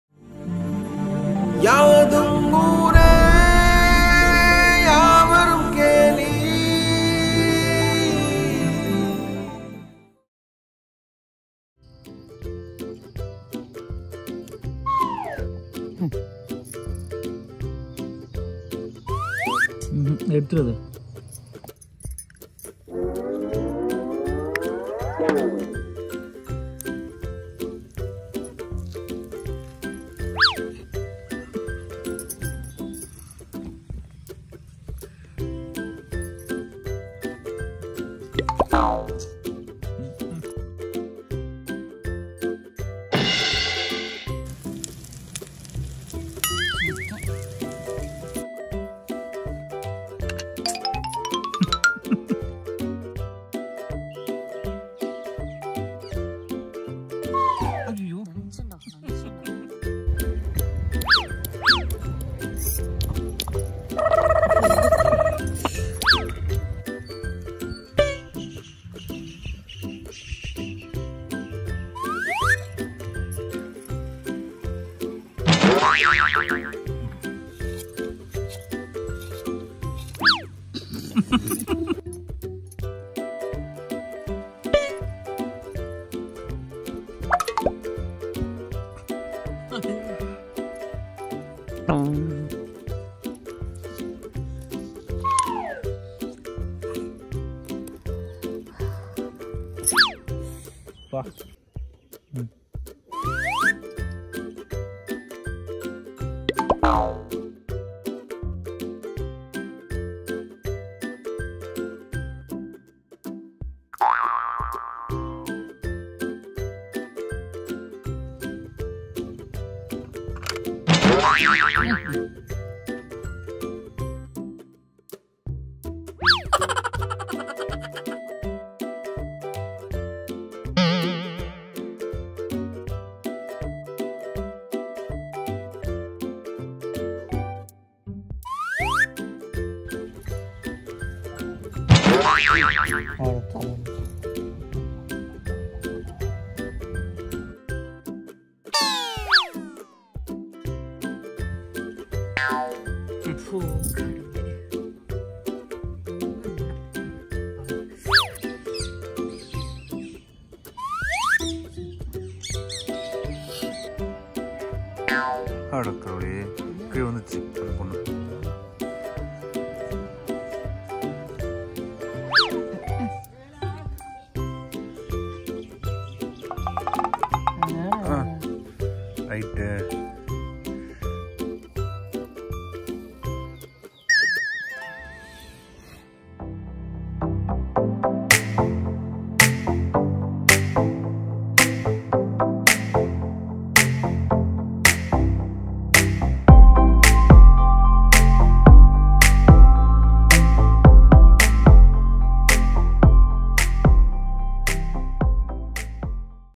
Bloopers_4 || The Tiny Foods
Hi My Dear Lovely Friends, Hope you all doing good

This is the video of collection of bloopers from our Latest Videos

This video includes the funny moments which happened while Ram & Valar preparing tiny foods, we hope you will enjoy_watch, have fun and smile :)

The Tiny Foods